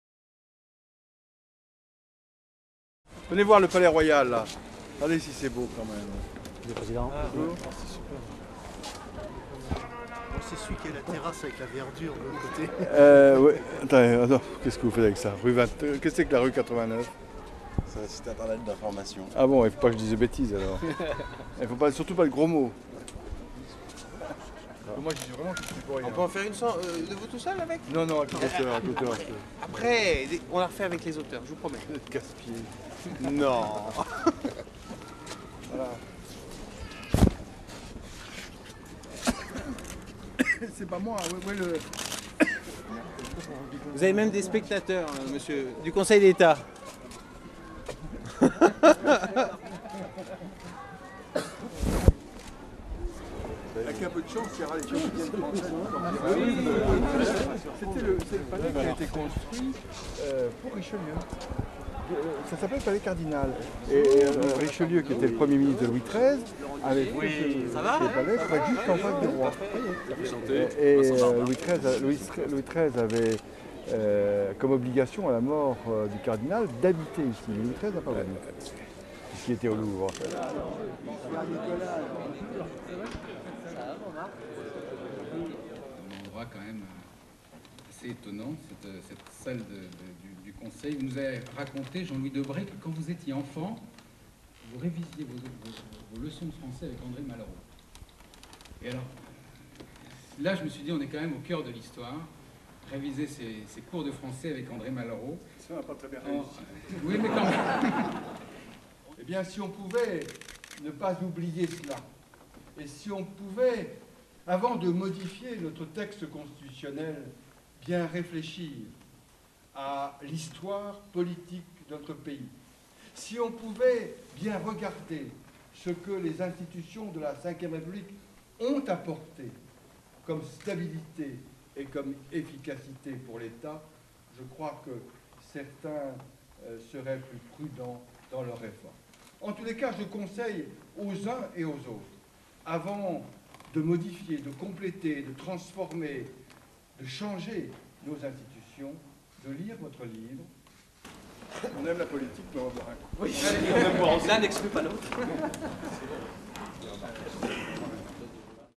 Debré et "La Ve République pour les nuls"
Le président du Conseil constitutionnel a présenté l'ouvrage, mercredi 12 mars.